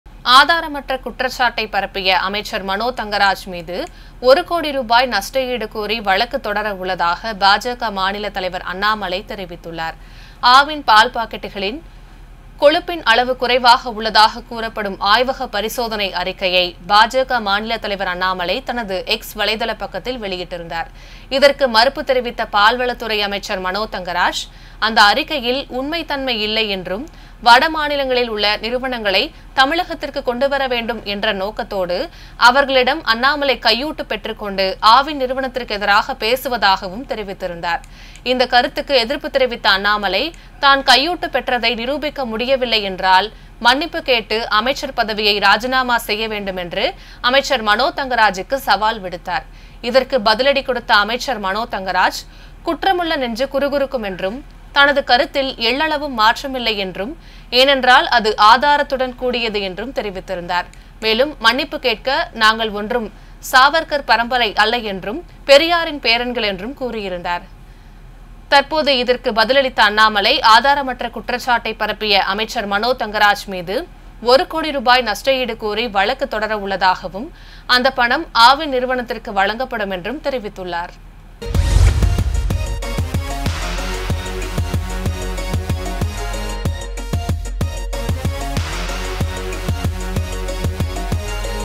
Annamalai | அமைச்சர் மனோ தங்கராஜ் மீது நஷ்ட ஈடு கோரி வழக்கு: அண்ணாமலை பேட்டி
ஆதாரமற்ற குற்றச்சாட்டைப் பரப்பிய அமைச்சர் மனோ தங்கராஜ் மீது ஒரு கோடி ரூபாய் நஷ்ட ஈடு கோரி வழக்கு தொடர உள்ளதாக பாஜக மாநிலத் தலைவர் அண்ணாமலை தெரிவித்துள்ளார்.